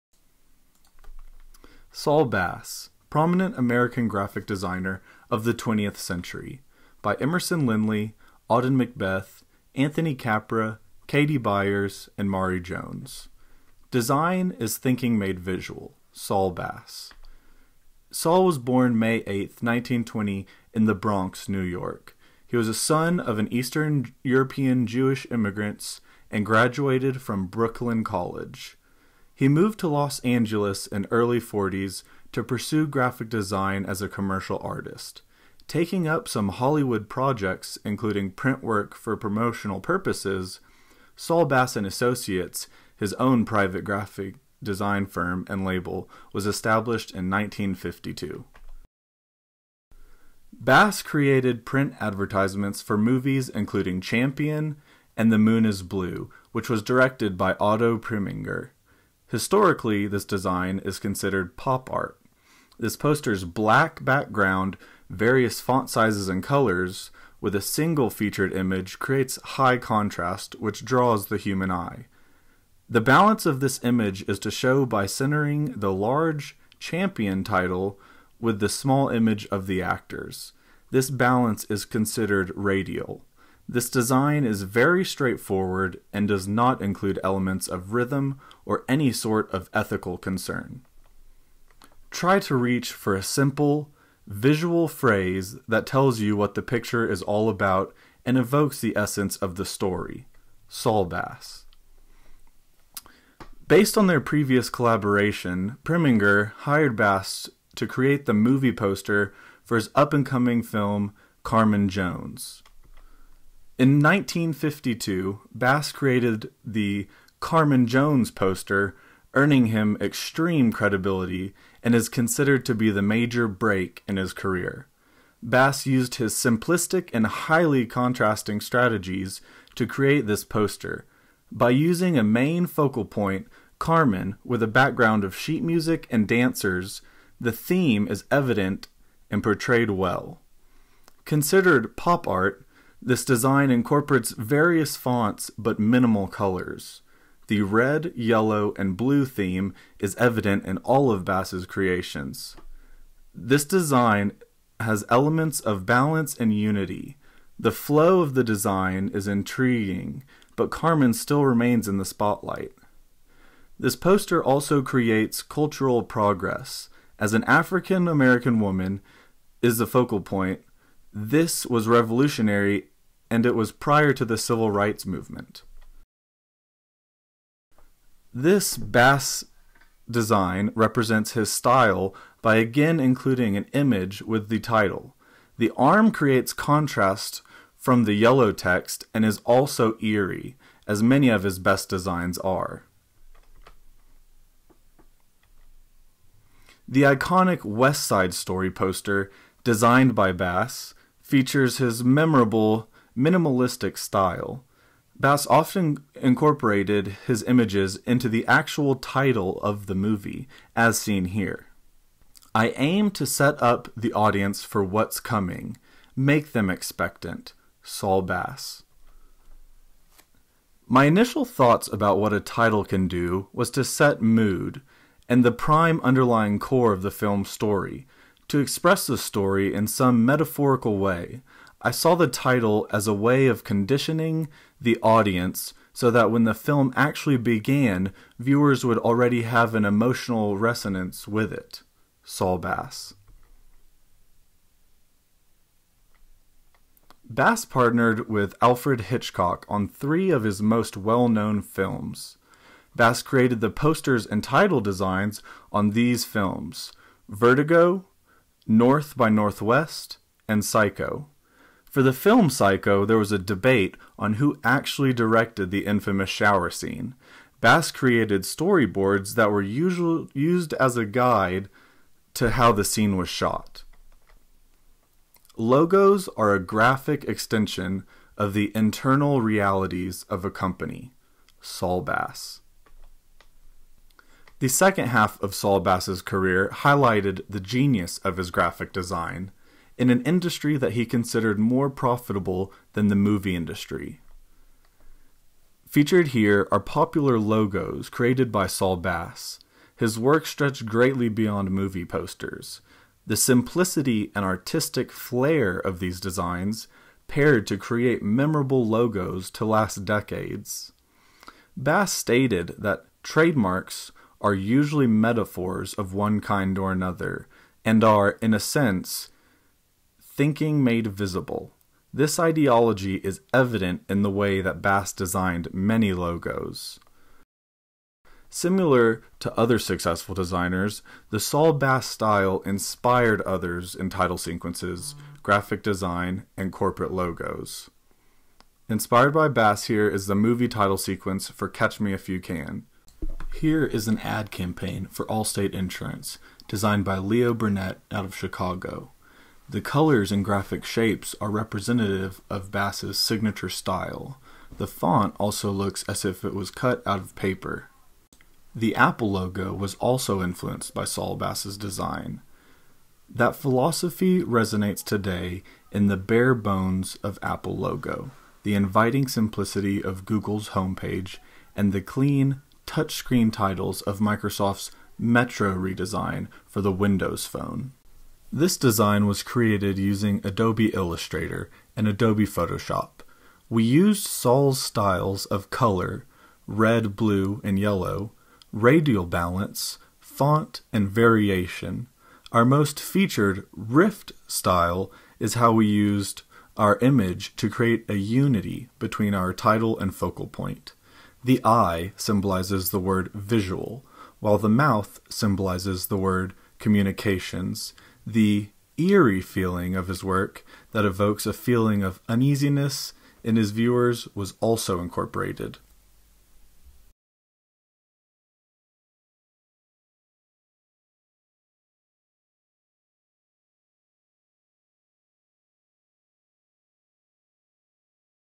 Saul Bass : Prominent American graphic designer of the twentieth-century
This video was created for a project in EMC 3308 Visual Communications at Texas Tech University.

Group members: Emerson Lindley, Auden McBeath, Anthony Capra, Katy Byers, and Marie Jones.